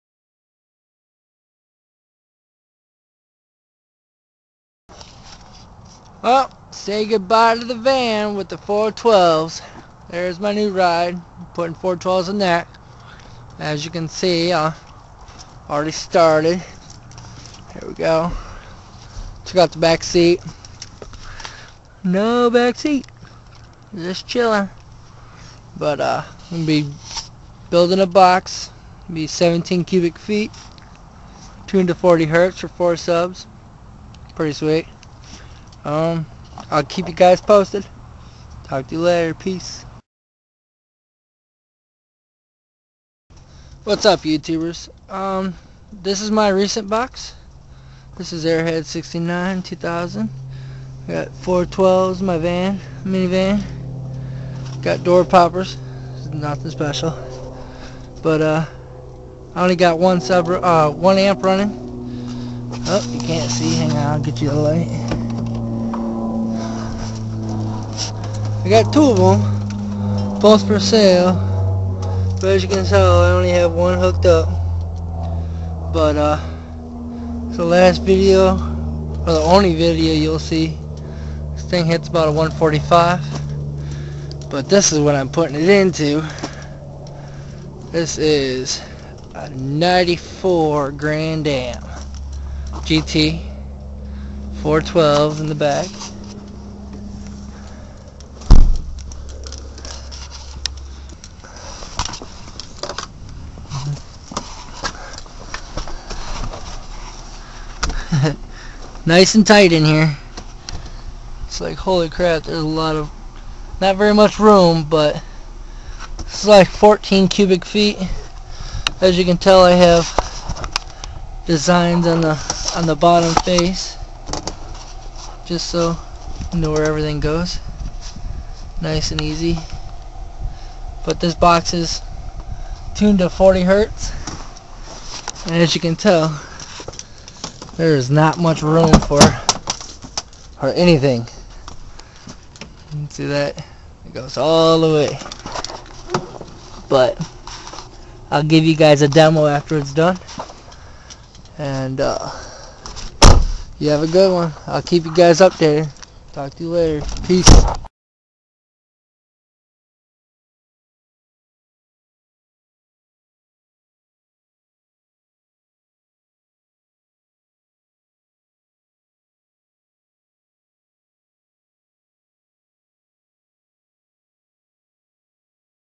FIRST BUILD VIDEO 1
first video of the build
4 12's in a 94 grand dam walled out